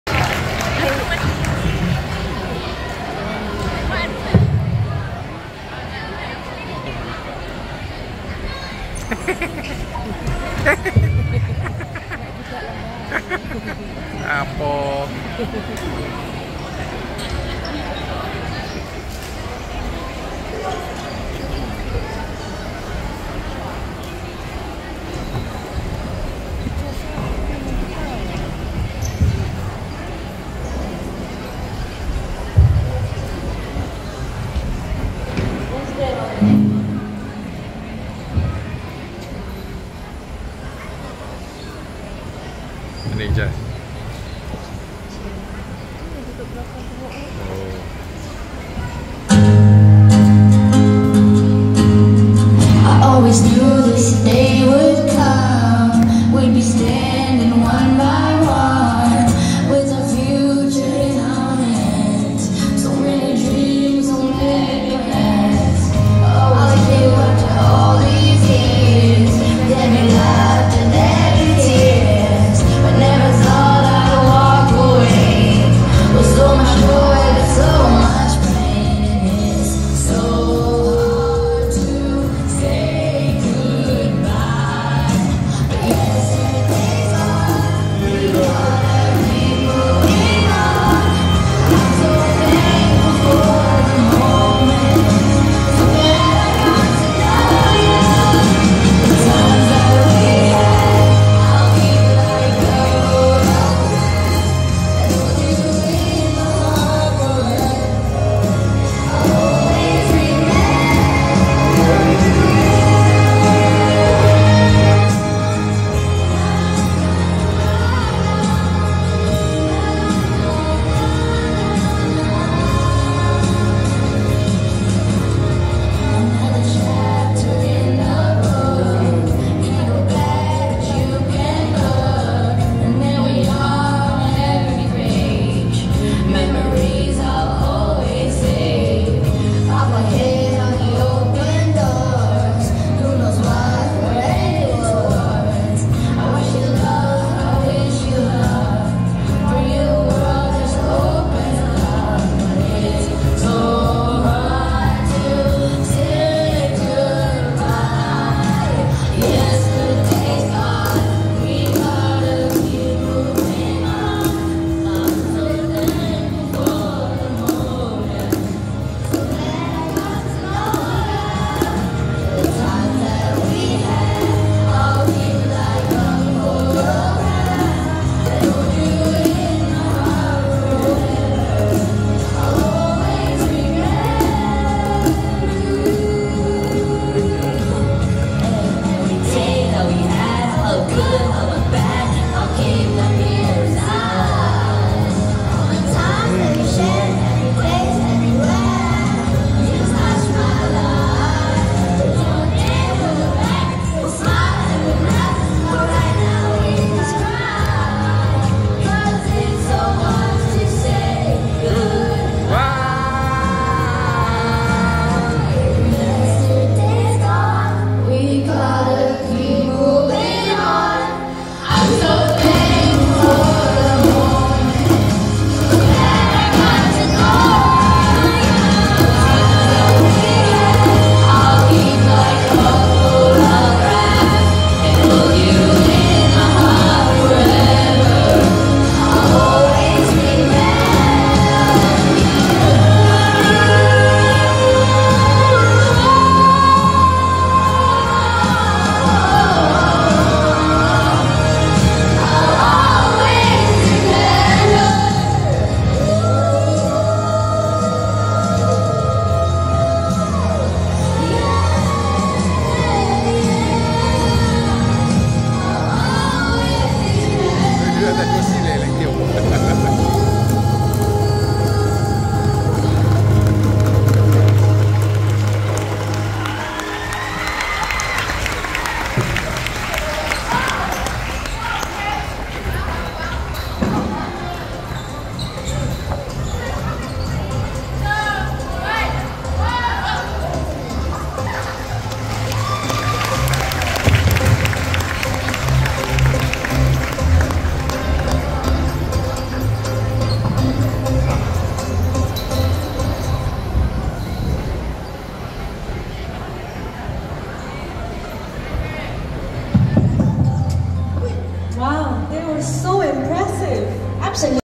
Class Graduation Performance by Primary 6G3 East Spring Primary School. 15th Nov2024 Fakhry_Hamza
His Class Graduation Performance Primary 6G3